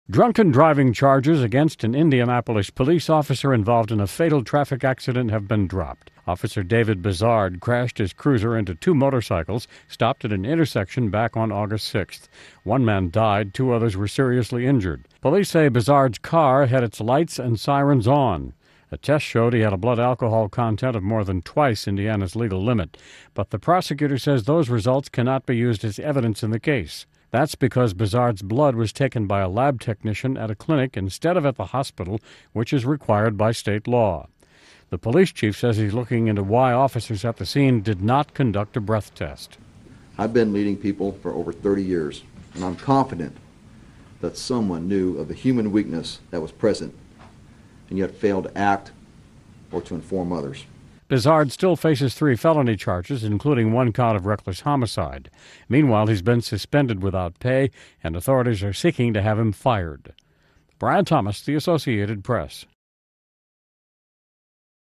DUI Charge Dropped Against Ind. Officer in Crash
A prosecutor said Thursday he is dropping drunken driving charges against an Indianapolis police officer accused in a crash that killed a motorcyclist because the officer's blood test wasn't properly obtained. (Aug. 19)